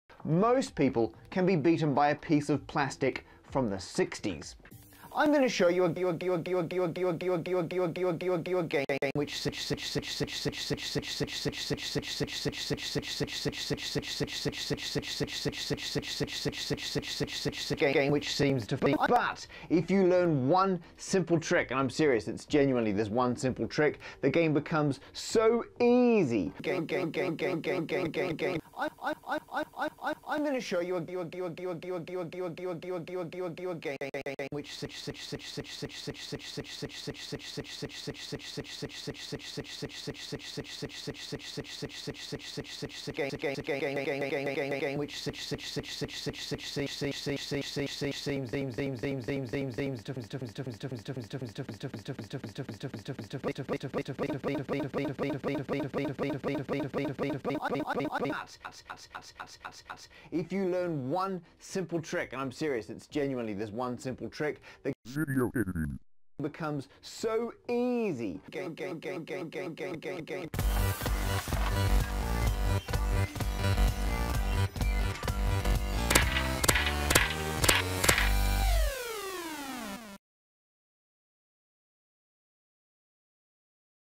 [YTP] WFMG Hates Camtasia Studio
This is why nobody approves of my memes. So many memes, I've had to work so hard to make, and it's all been taken fof granarg.

I have used the trial of Premiere Pro CC to make a few videos, and I've even got one unfinished project that I can't edit now, because I can't renew the trial. I just can't go back. This was a pain to make. It's just so expensive... I wish I could buy it in one go and have it forever.